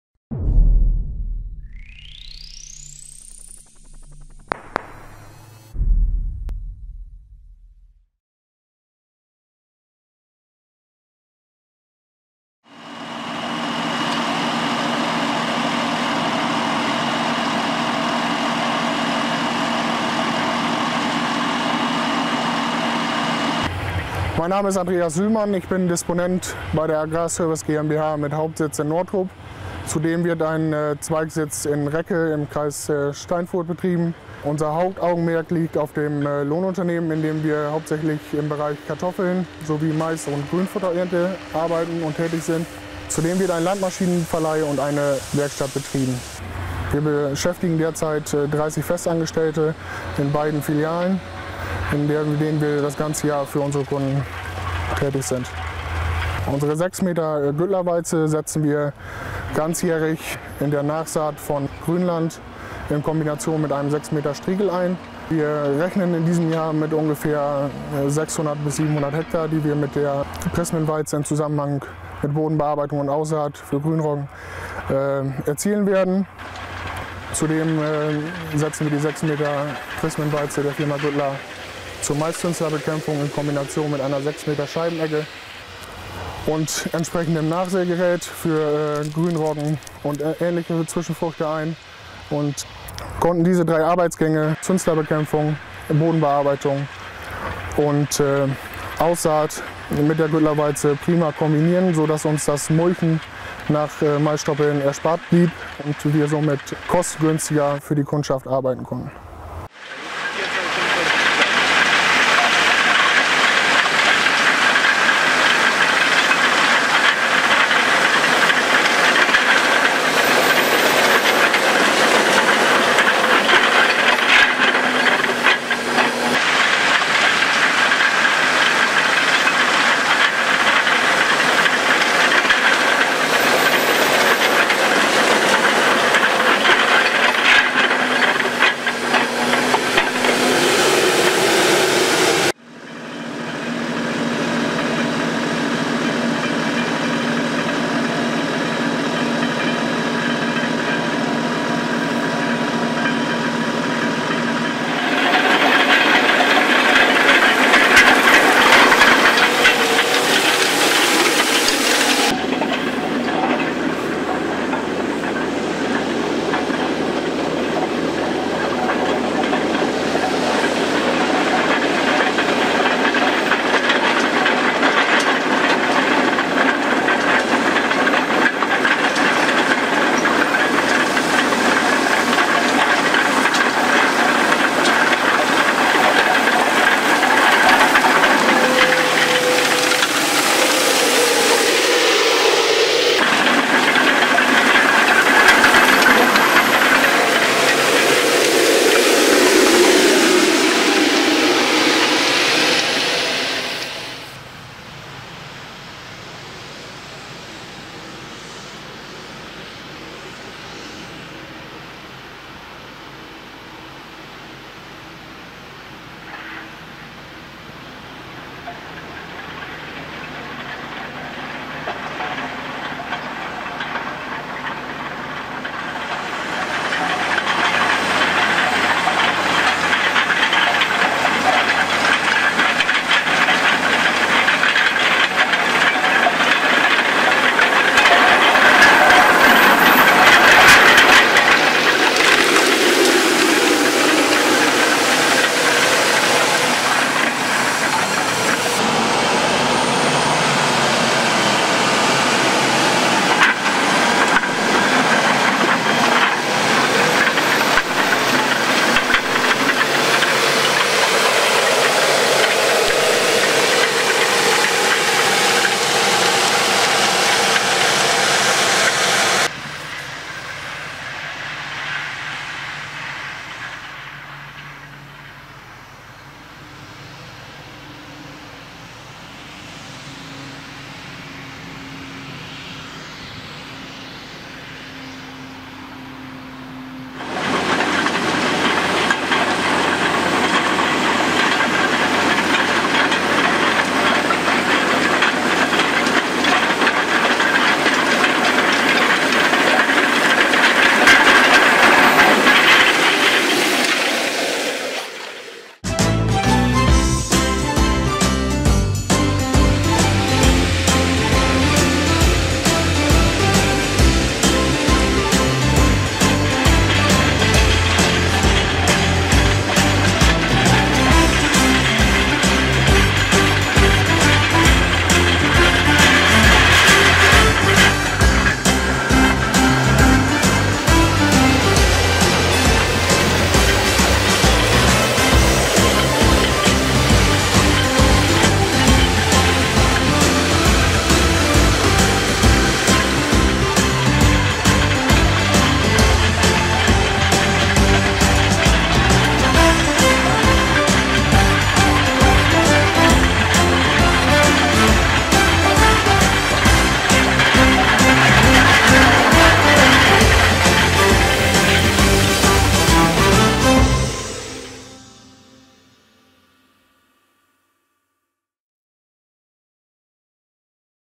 Güttler Matador 610 – Maiszünslerbekämpfung und Aussaat in einem Arbeitsgang
Die Matador 610 gingen zusammen mit dem GreenMaster 640 als Überraschungssieger aus dem TopAgrar Test zur Maiszünslerbekämpfung hervor. Durch den Anbau in der Front kann im Heck direkt mit der Bodenbearbeitung und auch mit der Aussaat begonnen werden. Die optionale Einebnungsschiene verbessert den Effekt zusätzlich, indem sie die Stängel vor der Walze umknickt.